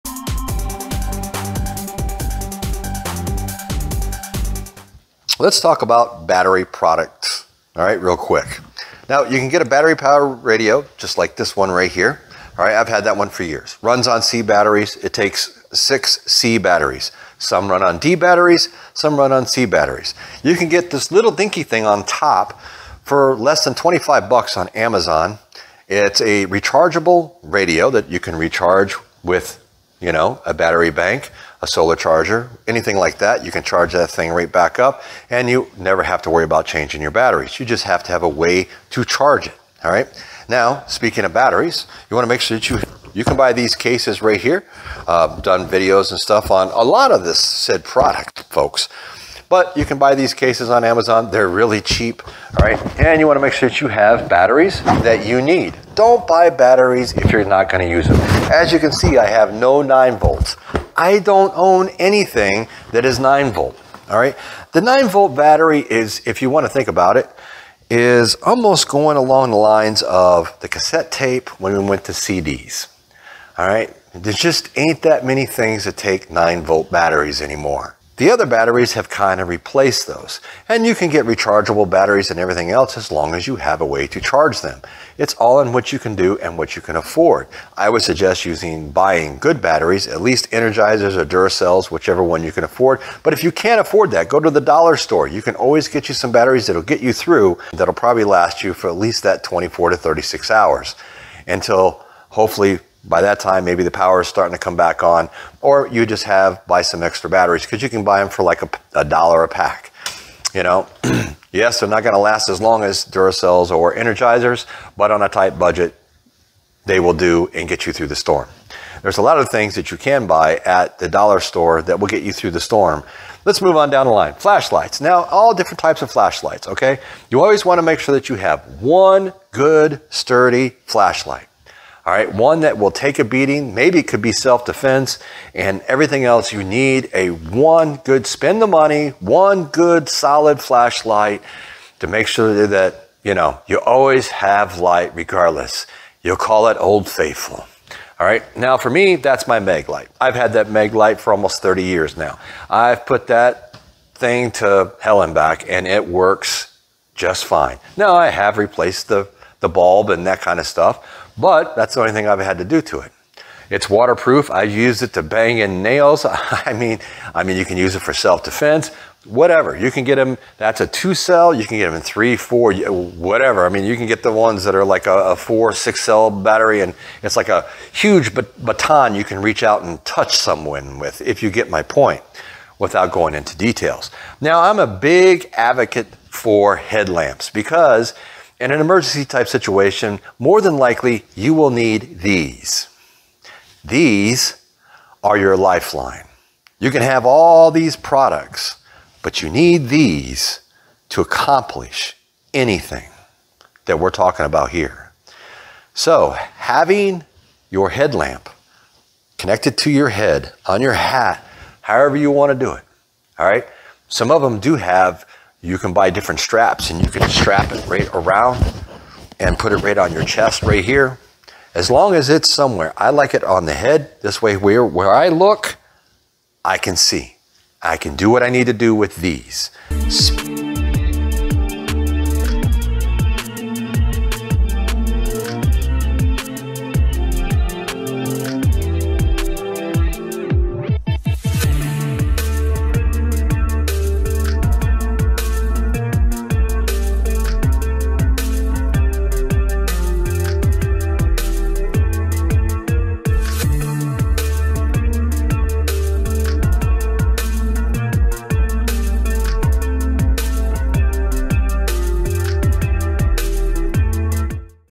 Quick Survival Tips over 7 days: Day 2 Radio's, Flashlights and Batteries
Quick Survival Tips in 4 minutes

FoodSaver FM5200 2-in-1 Automatic Vacuum Sealer